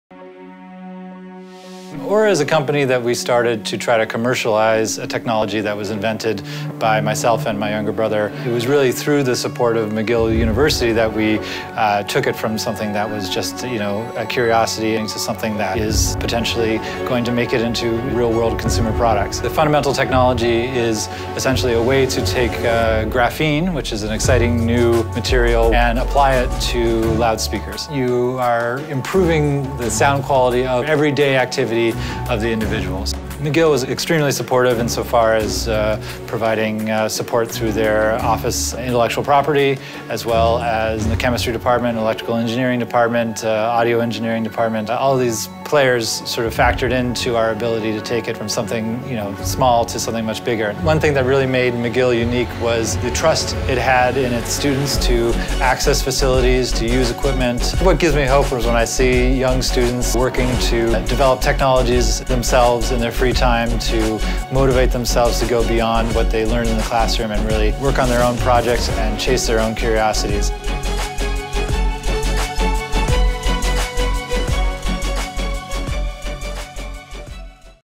Une idée née à McGill révolutionne notre perception du son
En menant leurs travaux à McGill, les frères Robert-Eric Gaskell, M. Mus. 2006, Ph. D. 2016, et Peter Gaskell, M. Ing. 2009, Ph. D. 2017, ont été frappés par l’inspiration : pourquoi ne pas utiliser le graphène – une matière dont la rigidité diminuerait la distorsion sonore et dont la légèreté optimiserait l’efficacité – pour concevoir le haut-parleur parfait? Grâce au soutien de McGill, dont celui du professeur Thomas Szkopek, qui les a conseillés et leur a donné accès aux installations, à l’appui du Bureau de la propriété intellectuelle et à une bourse d’innovation de 7 000 $, financée par un donateur de la Faculté de génie, les frères ont concrétisé leur idée et créé un produit qui pourrait révolutionner notre perception du son.